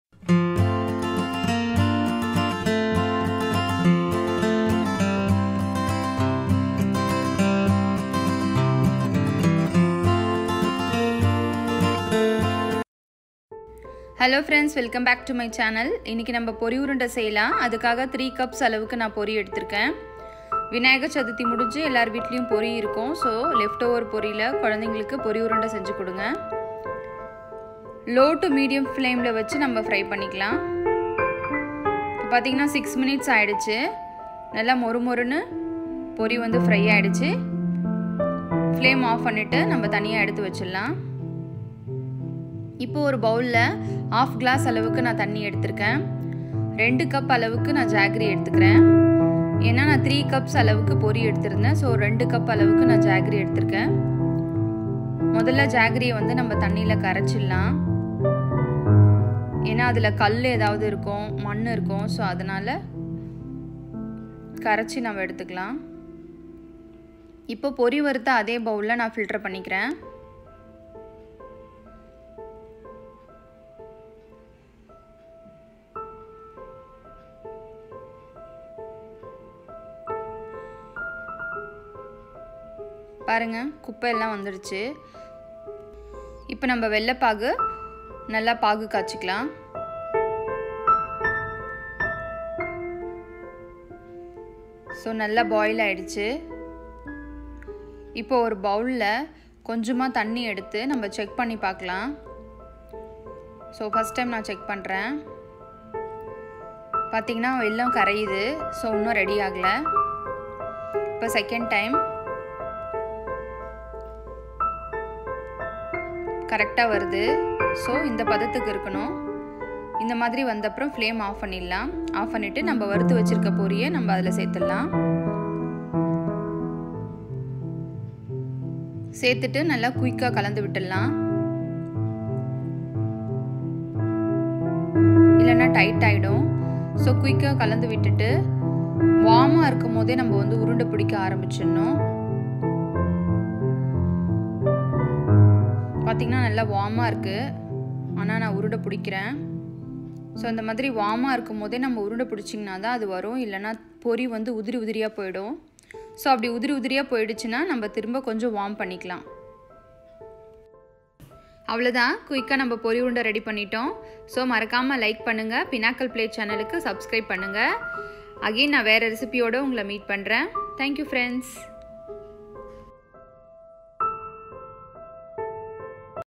Pori urundai/how to prepare pori urundai/puffed rice laddoo /evening snack recipe
Ingredients:

1.puffed rice-3 cup
2.jaggery-2 cup